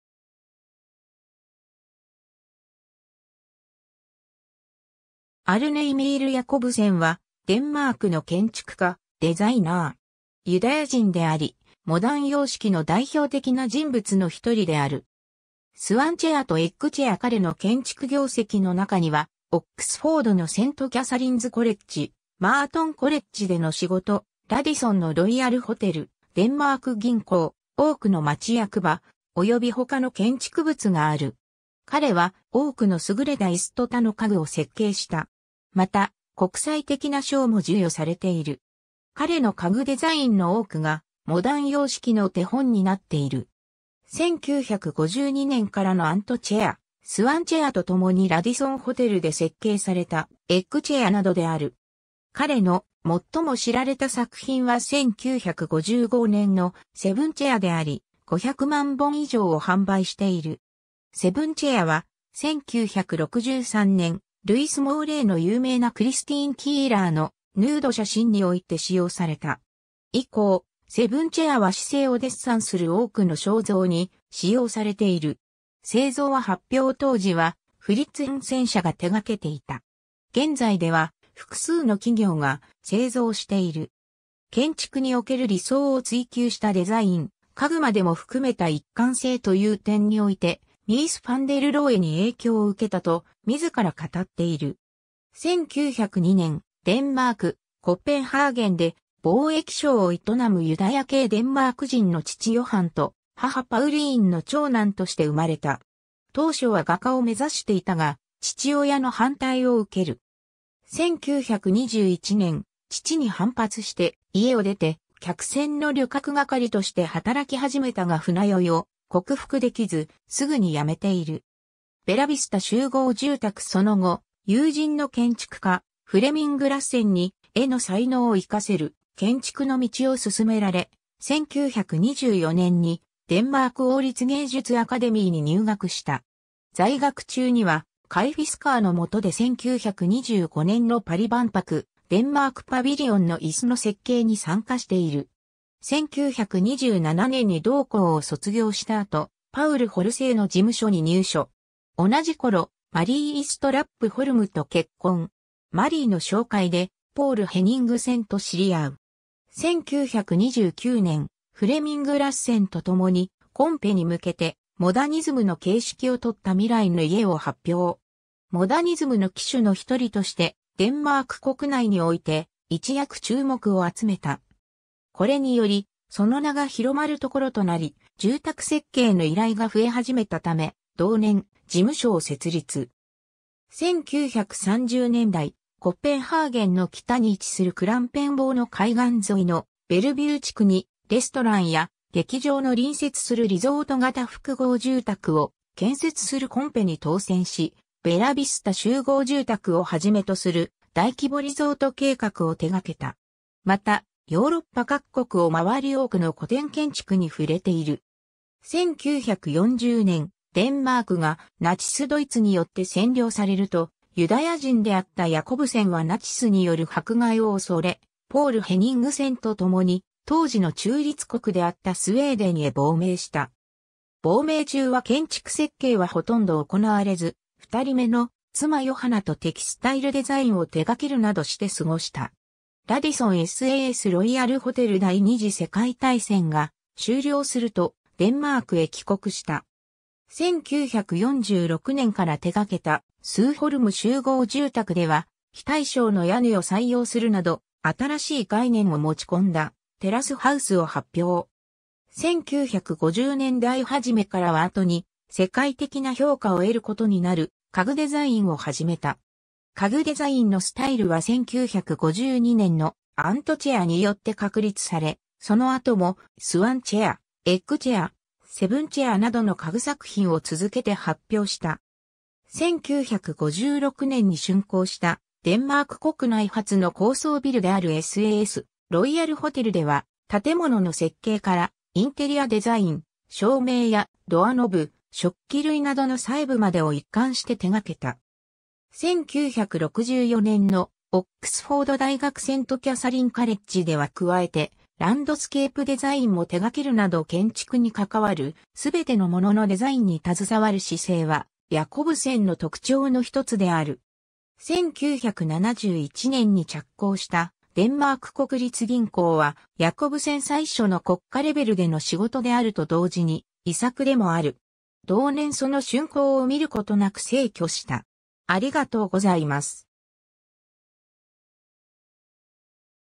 アルネ・ヤコブセン
アルネ・イミール・ヤコブセン（デンマーク語: Arne Emil Jacobsen、1902年2月11日 - 1971年3月24日）はデンマークの建築家、デザイナー。
ユダヤ人であり、モダン様式の代表的な人物の一人である。
 スワンチェア(右)とエッグチェア(左) 彼の建築業績の中には、オックスフォードのセントキャサリンズ・コレッジ、マートン・コレッジでの仕事、ラディソンのロイヤルホテル、デンマーク銀行、多くの町役場、および他の建築物がある。
 彼は多くの優れた椅子と他の家具を設計した。
 また、国際的な賞も授与されている。
 彼の家具デザインの多くがモダン様式の手本になっている。
1952年からのアントチェア、スワンチェアとともにラディソンホテルで設計されたエッグチェアなどである。
 彼の最も知られた作品は1955年のセブンチェアであり、500万本以上を販売している。
 セブンチェアは、1963年ルイス・モーレイの有名なクリスティーン・キーラーのヌード写真において使用された。
以降、セブンチェアは姿勢をデッサンする多くの肖像に使用されている。
製造は発表当時はフリッツ・ハンセン社が手がけていた。
現在では、複数の企業が、製造している。
 建築における理想を追求したデザイン、家具までも含めた一貫性と言う点においてミース・ファン・デル・ローエに影響を受けたと自ら語っている。
 1902年、デンマーク、コペンハーゲンで貿易商を営むユダヤ系デンマーク人の父ヨハンと母パウリーンの長男として生まれた。
当初は画家を目指していたが、父親の反対を受ける。
1921年、父に反発して家を出て、客船の旅客係として働き始めたが船酔いを克服できず、すぐに辞めている。
 ベラヴィスタ集合住宅 その後、友人の建築家、フレミング・ラッセンに絵の才能をいかせる建築の道を勧められ、1924年にデンマーク王立芸術アカデミーに入学した。
在学中にはカイ・フィスカーの下で1925年のパリ万博・デンマーク・パヴィリオンの椅子の設計に参加している。
1927年に同校を卒業した後、パウル・ホルセーの事務所に入所。
同じ頃、マリー・イストラップ・ホルムと結婚。
マリーの紹介でポール・ヘニングセンと知り合う。
 1929年、フレミング・ラッセンと共にコンペに向けてモダニズムの形式をとった未来の家を発表。
モダニズムの旗手の一人としてデンマーク国内において一躍注目を集めた。
これにより、その名が広まるところとなり、住宅設計の依頼が増え始めたため、同年、事務所を設立。
1930年代、コペンハーゲンの北に位置するクランペンボーの海岸沿いのベルヴュー地区に、レストランや劇場の隣接するリゾート型複合住宅を建設するコンペに当選し、『ベラヴィスタ集合住宅』をはじめとする大規模リゾート計画を手がけた。
またヨーロッパ各国を回り多くの古典建築に触れている。
 1940年、デンマークがナチス・ドイツによって占領されると、ユダヤ人であったヤコブセンはナチスによる迫害を恐れ、ポール・ヘニングセンと共に当時の中立国であったスウェーデンへ亡命した。
亡命中は建築設計は殆ど行われず、（事務所名義ではスタッフのみにより設計が行われている）二人目の妻ヨハナとテキスタイル・デザインを手がけるなどして過ごした。
 ラディソンSASロイヤルホテル 第2次世界大戦が終了すると、デンマークへ帰国した。
1946年から手がけた『スーホルム集合住宅』では非対称の屋根を採用するなど新しい概念を持ち込んだテラスハウスを発表。
1950年代初めからは後に世界的な評価を得ることになる家具デザインを始めた。
家具デザインのスタイルは1952年の『アント（蟻）チェア』によって確立され、その後も『スワンチェア』『エッグチェア』『セブンチェア』などの家具作品を続けて発表した。
1956年に竣工したデンマーク国内初の高層ビルであるSASロイヤルホテル（現・ラディソンブルーロイヤルホテル）では建物の設計からインテリアデザイン、照明やドアノブ、食器類などの細部までを一貫して手がけた。
1964年のオックスフォード大学セント・キャサリン・カレッジでは加えてランドスケープデザインも手がけるなど建築に関わるすべてのもののデザインに携わる姿勢はヤコブセンの特徴の一つである。
1971年に着工したデンマーク国立銀行はヤコブセン最初の国家レベル...